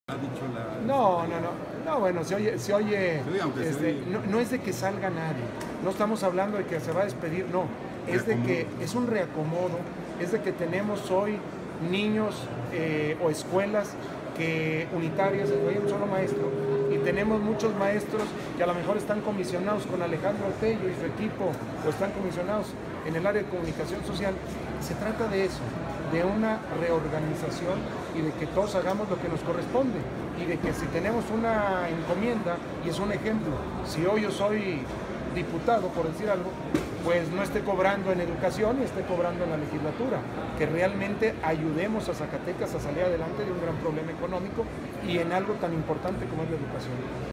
No se habla de despedir a nadie, es un reacomodo en el tema educativo: Tello
No es de que salga nadie, no hablamos de que se va a despedir, es un reacomodo, tenemos escuelas y maestros que están comisionados, se trata de una reorganización y de que todos hagamos lo que nos corresponde.